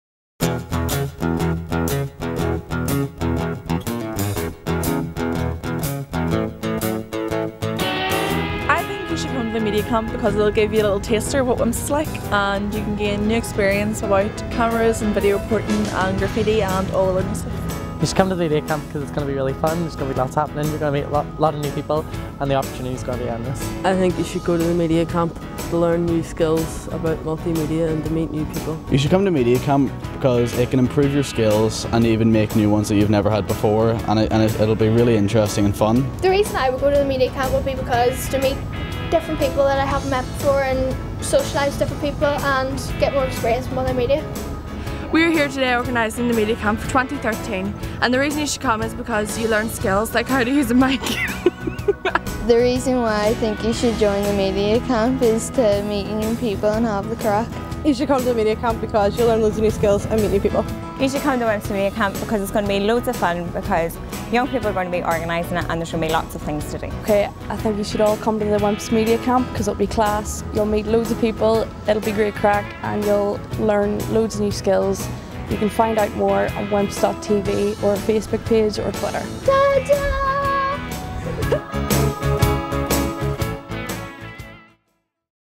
WIMPS.tv - WIMPS Media Camp 2013
From 19 to 23 August WIMPS Media Camp will bring together 100 young people to produce videos, audio, photographs, comics and more and report on the week through social media and video.

Find out more about how you can apply to take part at WIMPS.tv.